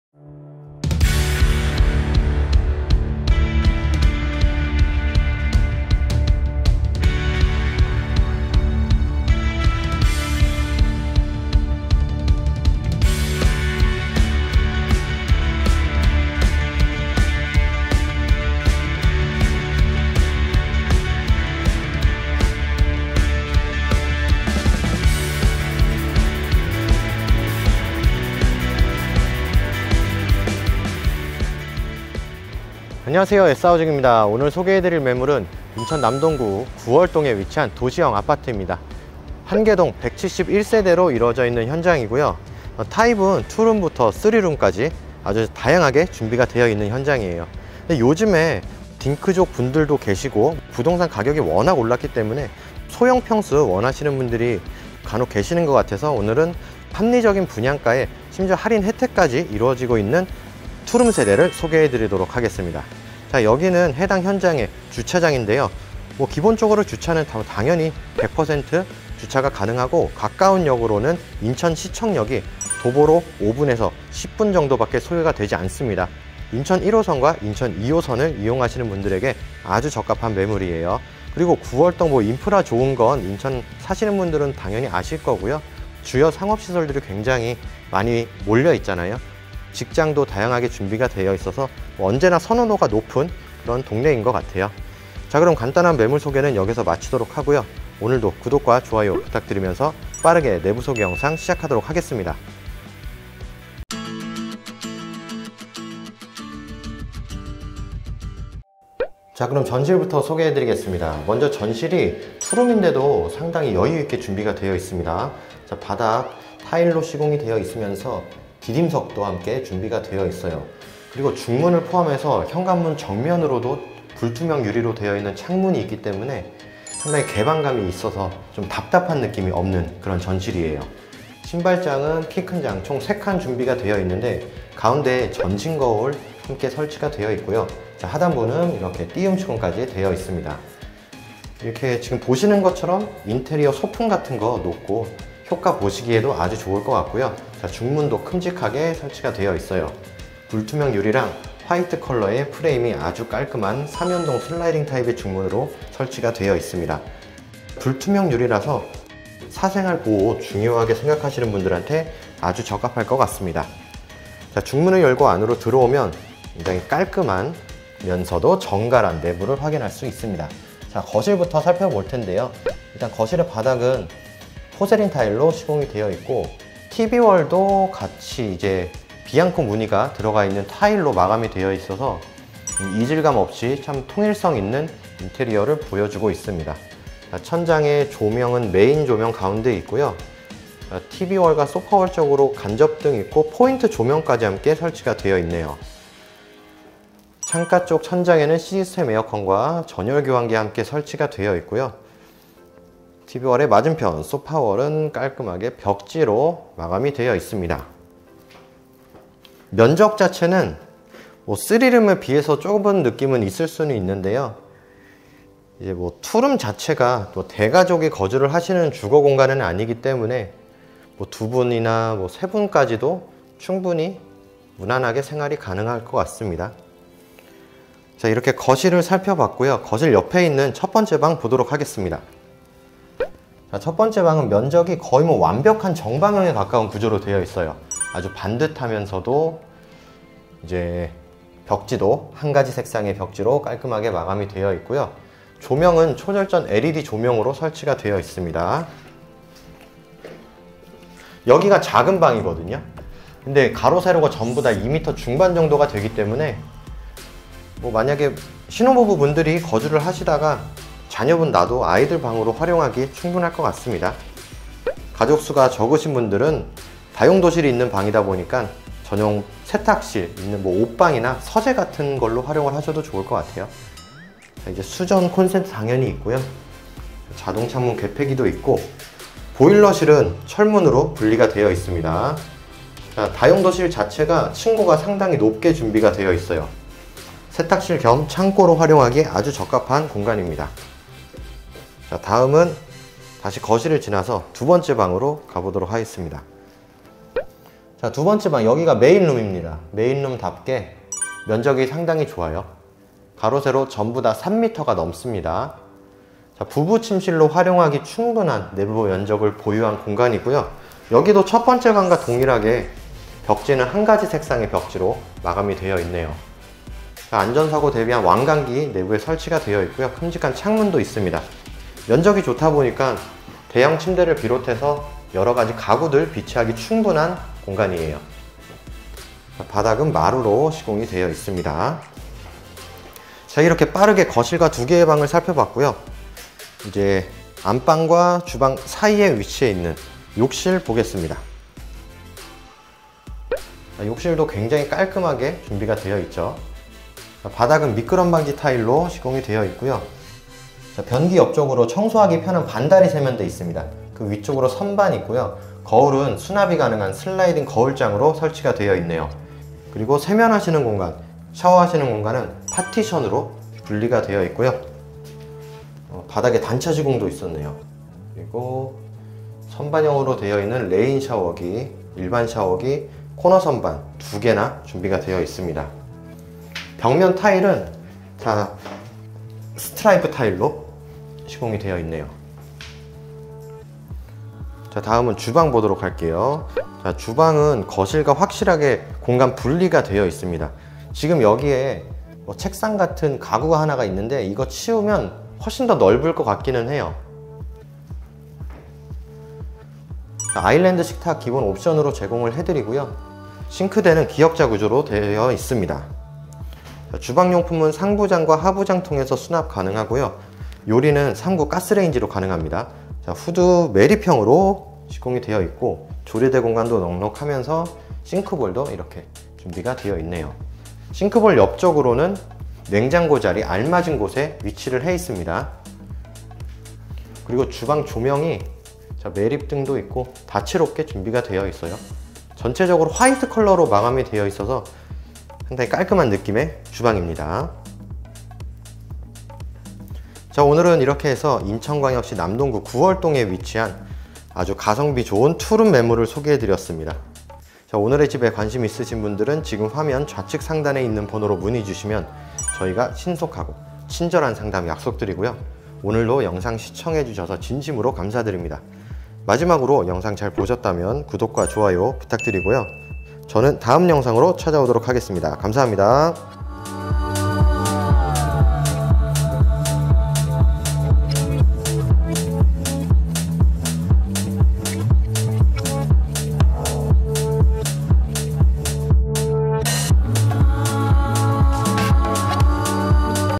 우리집 주변엔 없는게 없다?? 트렌디한 투룸 분양가 4000만원 할인! 인프라 좋고 교통까지 완벽한 인천신축빌라ㅣ구월동오피스텔ㅣ매물번호 857번
매물 번호를 말씀해 주시면 원활한 상담이 가능합니다^^

🏠 오늘의 인천 신축빌라 정보

✔ 매물번호_857번

✔ 소재지_인천광역시 남동구 구월동

✔ 주차_100%

✔ 분양면적_84.26㎡

✔ 인천시청역(인천1호선 & 인천2호선) 도보 5~10분

✔ 무풍 시스템에어컨

✔ 공기순환기

✔ 최고급 중문

✔ LED 인테리어 조명

✔ 분양가 2억 초반

★ 다양한 신축빌라 정보가 가득 담긴 한현준 팀장 블로그 ★

▶ Chapter

00:00 Opening
00:32 Information
01:48 Entrance
02:51 Living room
03:53 Room 1
04:57 Room 2
05:43 Bathroom
06:25 Kitchen
07:20 Outro